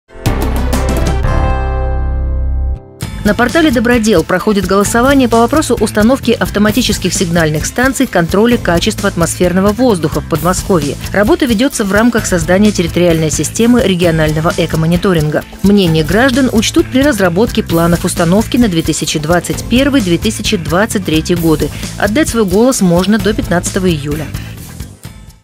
Голосуем на «Доброделе» за чистый воздух
На портале «Добродел» проходит голосование по вопросу установки автоматических сигнальных станций контроля качества атмосферного воздуха в Подмосковье. Работа ведется в рамках создания территориальной системы регионального экомониторинга.